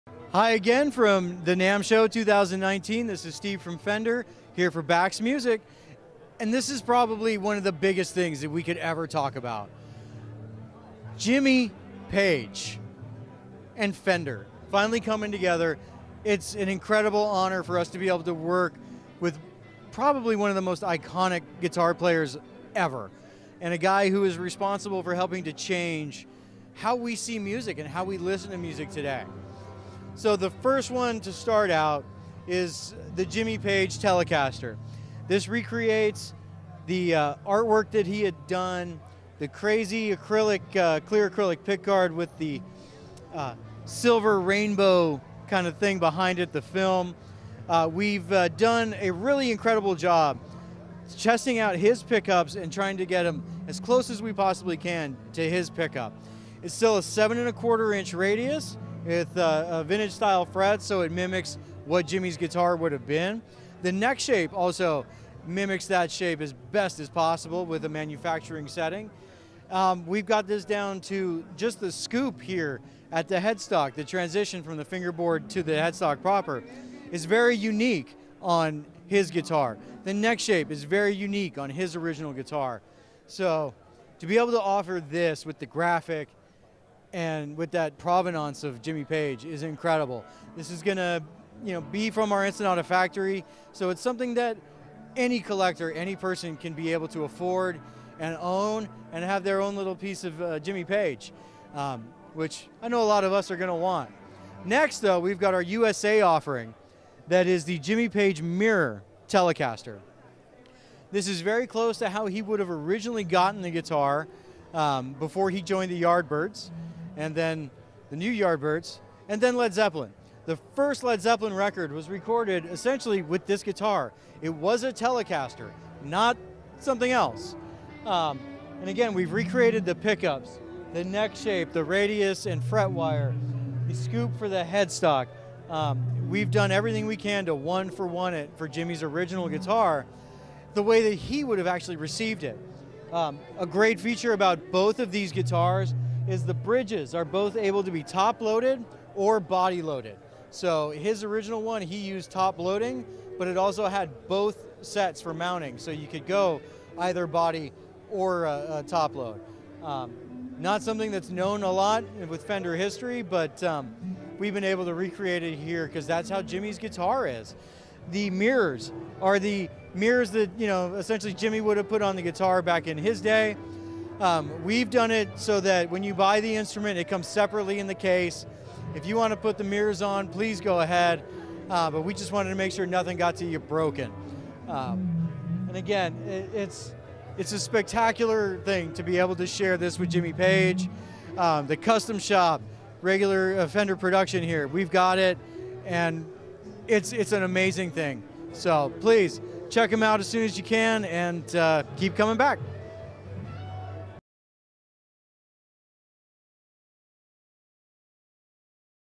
Fender Jimmy Page Signature Telecasters | NAMM 2019
De Telecaster van Jimmy Page staat aan de basis van vele iconische riffs en is niet meer weg te denken uit de rockgeschiedenis. De legende duurt voort in de vorm van de Fender Jimmy Page Signature Telecaster met authentieke Dragon-afwerking! Meer info